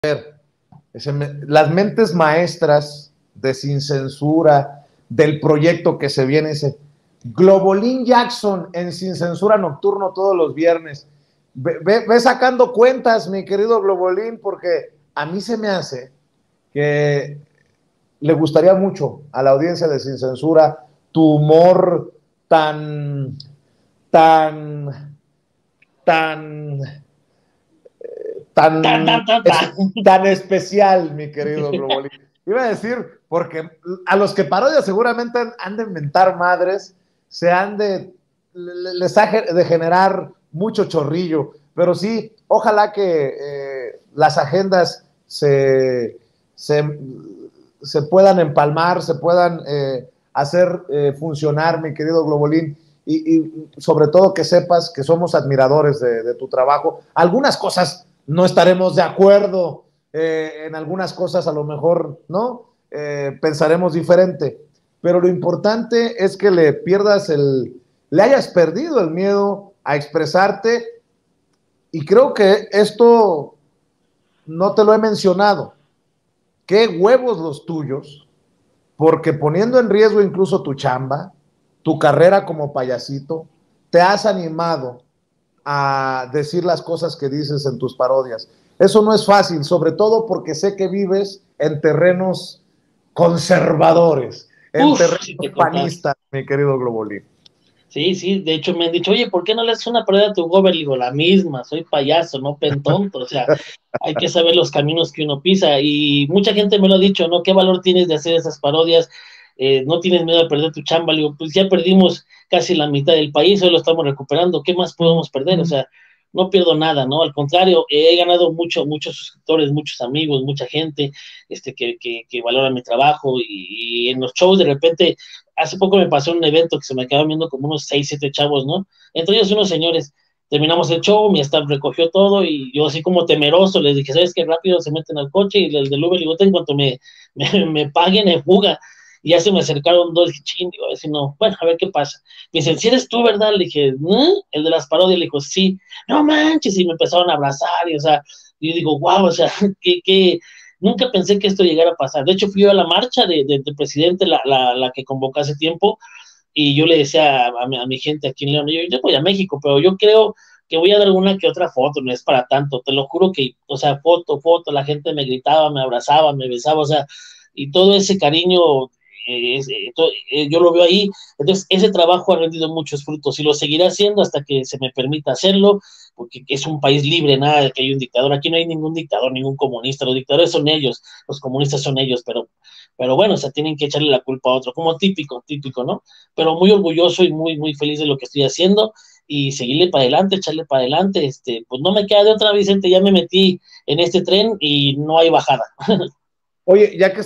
¡QUÉ HU3V0T3S LOS DE GLOBOLIN JACKSON! PARODIA DURO A LOS MÁS PODEROSOS
¡Qué hu3v0t3s los de Globolin Jackson al parodiar a los más poderosos! El payaso profesional nos comenta que una vez hasta le dio angustia que unas personas se le quedaban viendo, pero que después le dijeron que lo querían conocer por sus parodias.